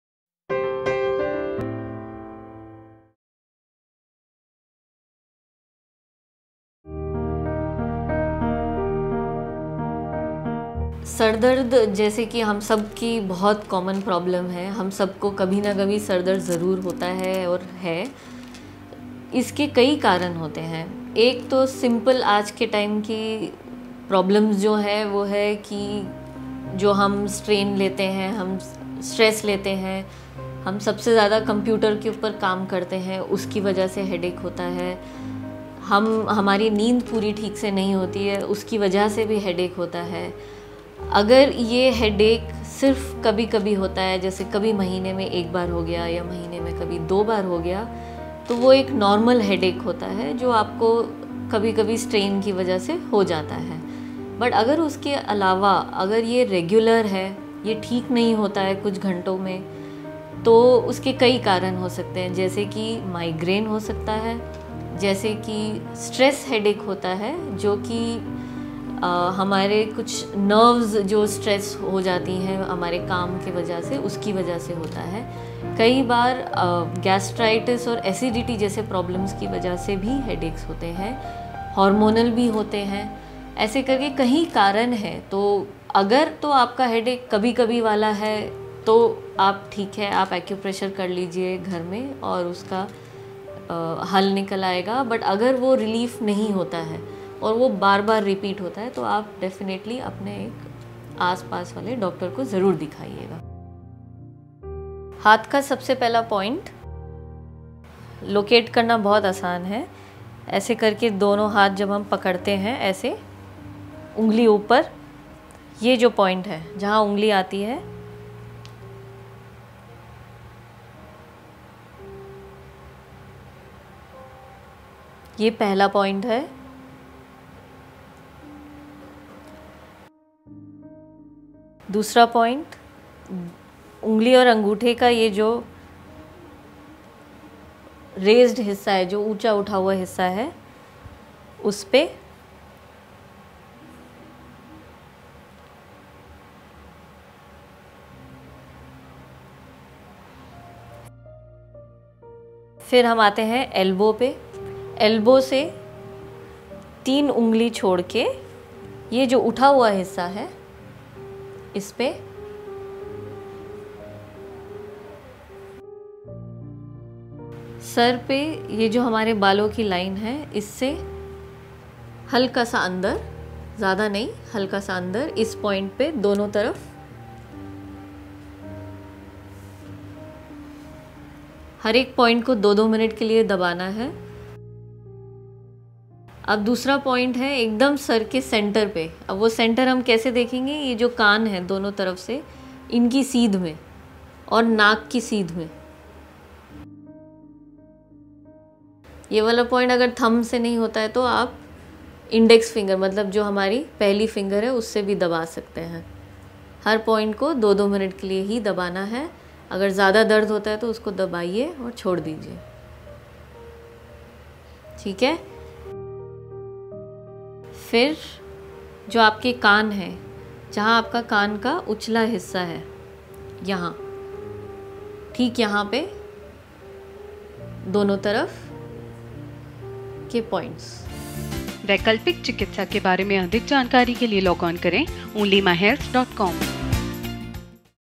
सर दर्द के लिए एक्यूप्रेशर - Onlymyhealth.com
वर्तमान लाइफस्‍टाइल में सरदर्द की समस्‍या एक सामान्‍य समस्‍या हो गई है। सभी आये दिन इस समस्‍या से अपने-आप ग्रस्‍त हो जाते हैं। इसके लिए सबसे अधिक जिम्‍मेदार हमारा तनाव लेना होता है। इसके अलावा नींद न पूरी होना और लगातार कंप्‍यूटर पर काम करने के कारण भी सरदर्द होता है। अगर आपको कभी-कभी यानी महीने में एक बार सरदर्द होता है तो यह नॉर्मल है। लेकिन अगर यह नियमित रूप से हो रहा है तो यह माइग्रेन या नर्व में संकुचन के कारण भी हो सकता है। अगर कभी-कभी होने वाला सरदर्द है तो एक्‍यूप्रेशर के जरिये इसपर काबू पाया जा सकता है। इसके लिए हाथ का सबसे पहला प्‍वाइंट कलाई के पास और दूसरा प्‍वाइंट उंगली के ऊपर उठे हुए हिस्‍से पर और तीसरा प्‍वाइंट एल्‍बो के पास उठे हुए हिस्‍से पर, चौथा प्‍वाइंट सर पर बालों की लाइन से थोड़ा सा अंदर वाले हिस्‍से पर। इसके अलावा कई दूसरे प्‍वाइंट भी हैं जिसे इस वीडियो में हमने दिखाया है। इन बिंदुओं पर 2 मिनट तक के लिए प्रेशर दीजिए, इससे सरदर्द से आराम मिल जायेगा।